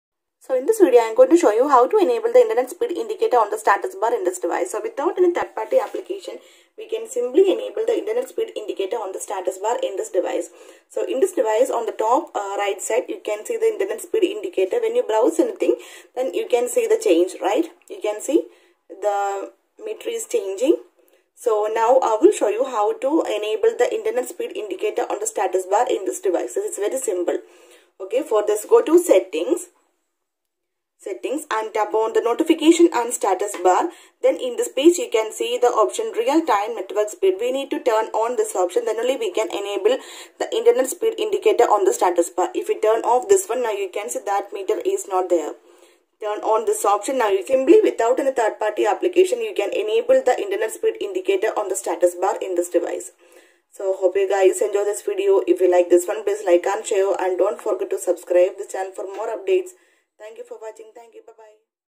Realme X7 MAX | How To Enable Internet Speed Indicator on Status Bar
Hi friends this video shows you that " how to enable internet speed indicator o status bar in Realme X7 MAX "